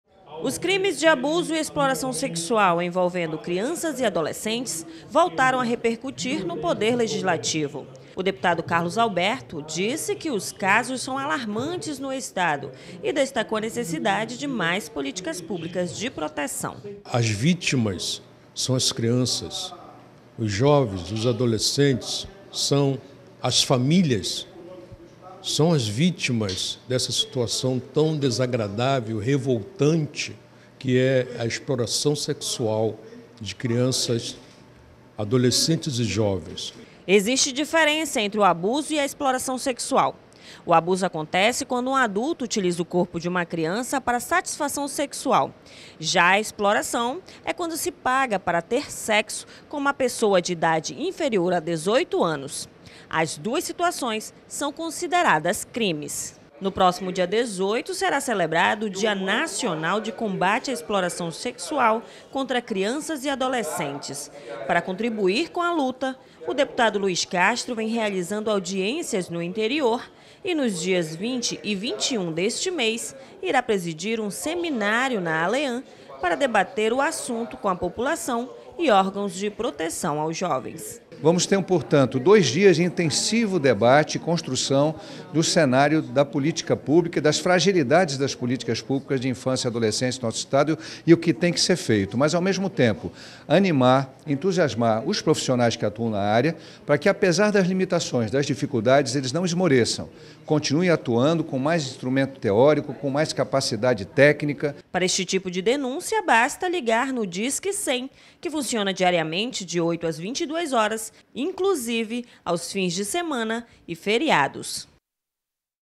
Combate ao abuso de menores é defendido no poder legislativo
Os parlamentares defenderam mais políticas públicas que protejam crianças e adolescentes de crimes de violação sexual. O deputado Carlos Alberto, alertou para os casos preocupantes no amazonas.

Repórter: Priscila Senna